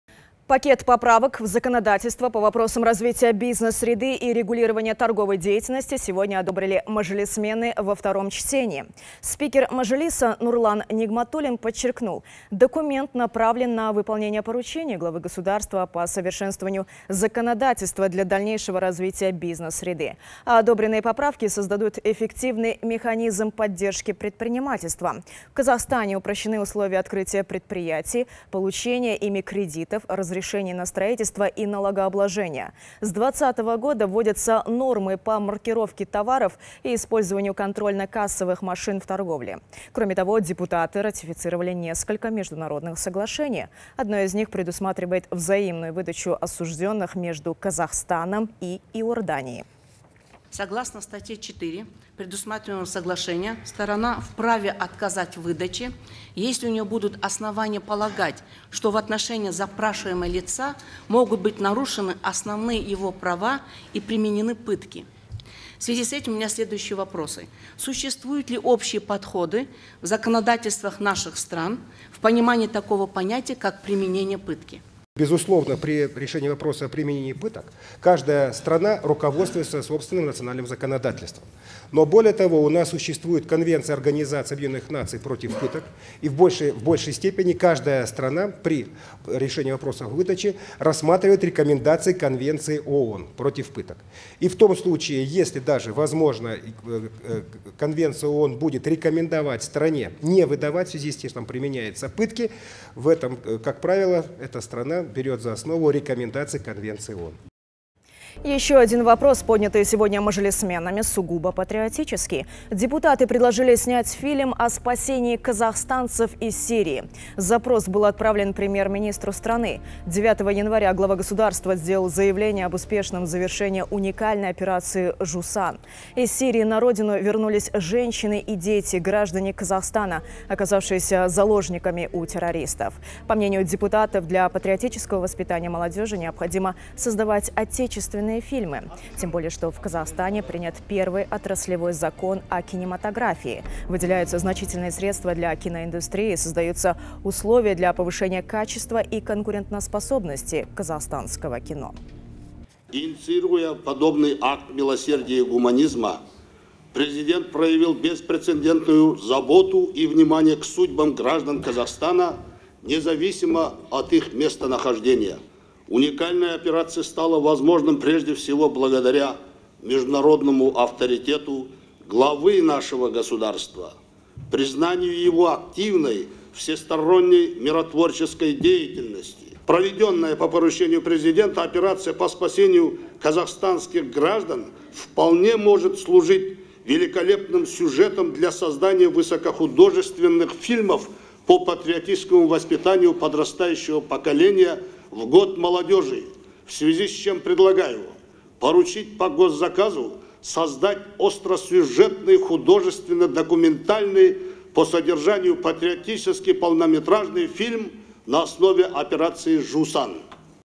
Мажилис одобрил ряд новых законопроектов
Пакет поправок в законодательство по вопросам развития бизнес-среды и регулирования торговой деятельности сегодня одобрили мажилисмены во II чтении.

Спикер Мажилиса Нурлан Нигматулин подчеркнул: документ направлен на выполнение поручений Главы государства по совершенствованию законодательства для дальнейшего развития бизнес-среды. А одобренные поправки создадут эффективный механизм поддержки предпринимательства.

В Казахстане упрощены условия открытия предприятий, получения ими кредитов, разрешений на строительство и налогообложения. С 2020 года вводятся нормы по маркировке товаров и использованию контрольно-кассовых машин в торговле. Кроме того, депутаты ратифицировали несколько международных соглашений. Одно из них предусматривает взаимную выдачу осужденных между Казахстаном и Иорданией.